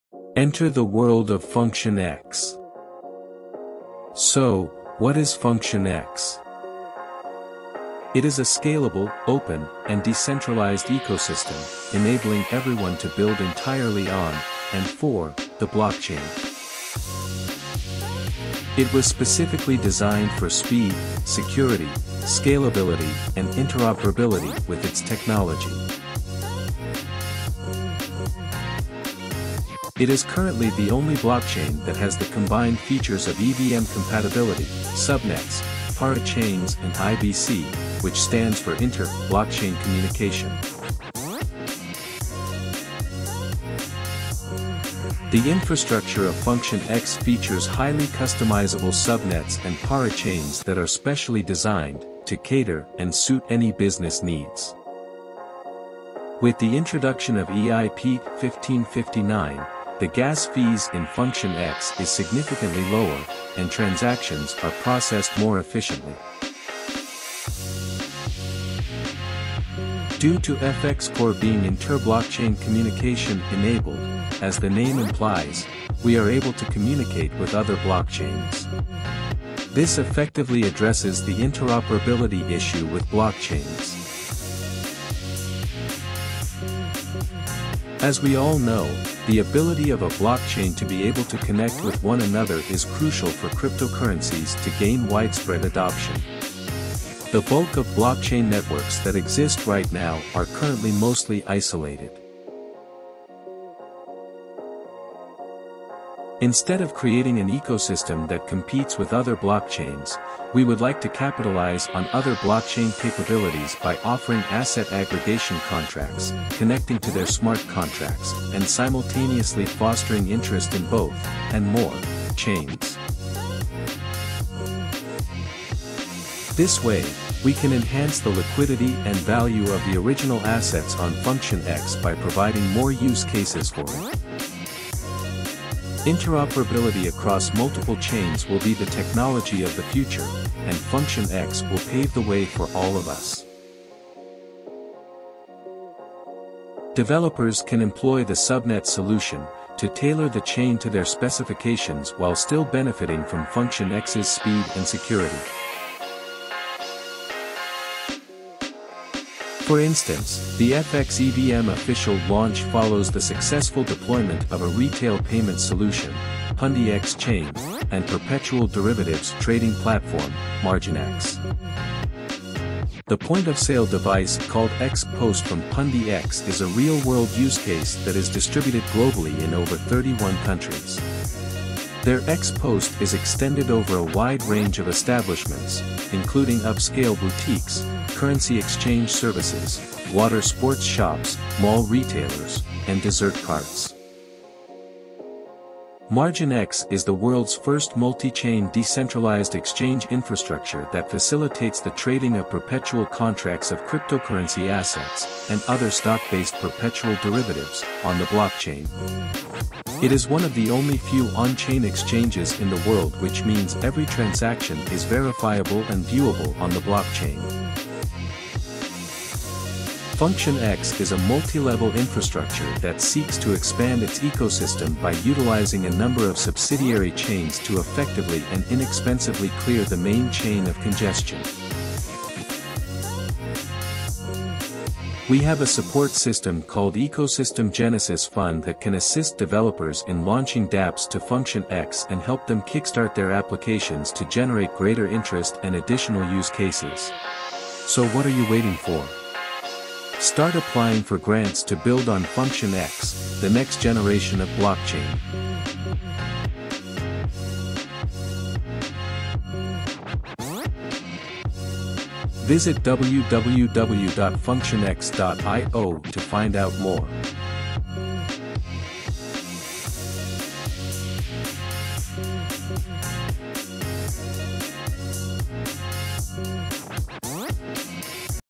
Introducing Function X - The Next Generation Blockchain
Introducing Function X to the world.

Developers can start porting DApps over to the Function X Ecosystem and apply for grants to jumpstart your project and generate interest and create additional use cases.

Function X is a scalable, open, and decentralized ecosystem, enabling all to build entirely on, and for, the blockchain.

The primary objective of Function X is not to outclass other blockchains but rather to complement them by capitalizing on their advantages, such as the ability to connect to Ethereum smart contracts, offer asset aggregation contracts, and raise awareness of both (and additional) chains simultaneously.

FX Features:
✅ EVM Compatible
✅ Subnet / Parachain
✅ Multi-Faceted Nature (Highly customizable for different business needs)
✅ Inter-Blockchain Communication (IBC)
✅ Decentralized
✅ Global Validators
✅ High Throughput
✅ Low Gas Fees
✅ 69.5% Staked Ratio ( 24% Staking APY )

When companies like Function X start thinking of complimenting other blockchains instead of fighting for the number 1 spot, progress and productivity moves at a much faster pace since it is meant to benefit all parties all around.

There is a saying, "One For All, All For One".

Highly customizable to suit your business needs. A blockchain solution built for everyone.

It is also the first blockchain to launch its own subnet - PundiXChain on October 13th 2021 - A payment-focused blockchain in the Function X Ecosystem, featured with low latency and low transaction fees.

The second subnet is MarginX - They are the world’s first multi-chain DEX that facilitates the trading of cryptocurrency perpetuals and stock-based derivatives on the blockchain.

𝐄𝐑𝐂𝟐𝟎 𝐀𝐝𝐝𝐫𝐞𝐬𝐬 → 0xB4A66887FE5ccc7f79D4aB3e48Af977051a74b1e
𝐅𝐗𝐂𝐨𝐫𝐞 𝐀𝐝𝐝𝐫𝐞𝐬𝐬 → 0xB4A66887FE5ccc7f79D4aB3e48Af977051a74b1e

Every little bit helps. Thank You With Love! 🥰